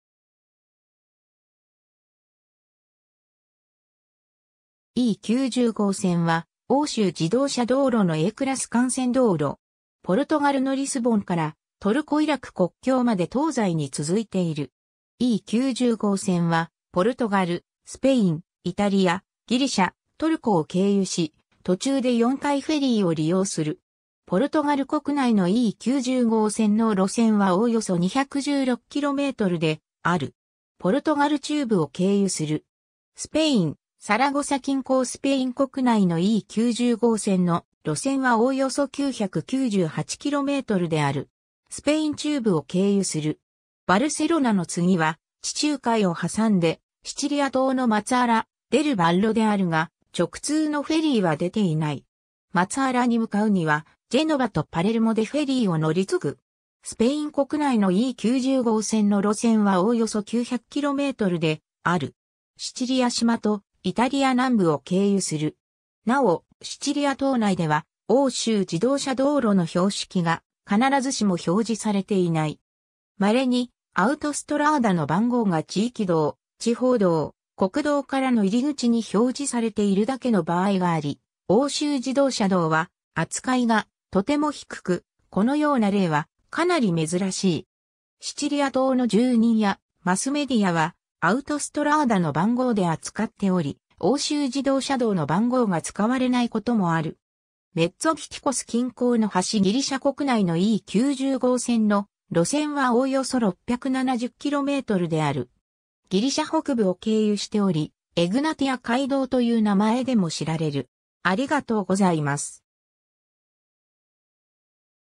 E90号線
E90号線（E90ごうせん、英語: European route E90）は欧州自動車道路のAクラス幹線道路。
ポルトガルのリスボンからトルコ・イラク国境まで東西に続いている。
 E90号線はポルトガル、スペイン、イタリア、ギリシャ、トルコを経由し、途中で4回フェリーを利用する。
 ポルトガル国内のE90号線の路線はおおよそ216kmである。
ポルトガル中部を経由する。
 スペイン、サラゴサ近郊 スペイン国内のE90号線の路線はおおよそ998kmである。
スペイン中部を経由する。
 バルセロナの次は地中海を挟んでシチリア島のマツァーラ・デル・ヴァッロであるが、直通のフェリーは出ていない。
マツァーラに向かうにはジェノヴァとパレルモでフェリーを乗り継ぐ。
 スペイン国内のE90号線の路線はおおよそ900kmである。
シチリア島とイタリア南部を経由する。
 なお、シチリア島内では欧州自動車道路の標識が必ずしも表示されていない。
まれに、アウトストラーダの番号が地域道、地方道、国道からの入り口に表示されているだけの場合があり、欧州自動車道は扱いがとても低く、このような例はかなり珍しい。
シチリア島の住人やマスメディアはアウトストラーダの番号で扱っており、欧州自動車道の番号が使われないこともある。
 Metsovitikos近郊の橋 ギリシャ国内のE90号線の路線はおおよそ670kmである。
ギリシャ北部を経由しており、エグナティア街道という名前でも知られる。